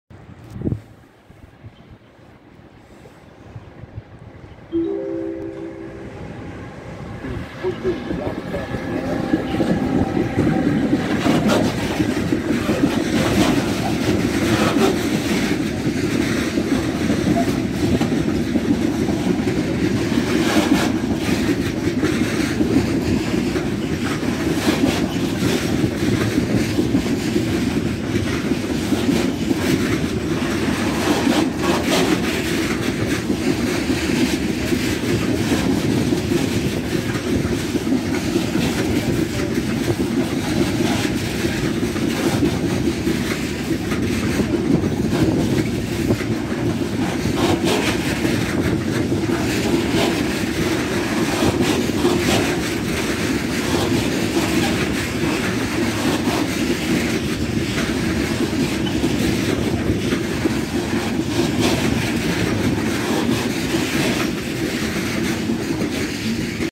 Řada 193 Pardubice
Čau dnes tu máme Vectrona společnosti Budmar group v Pardubicích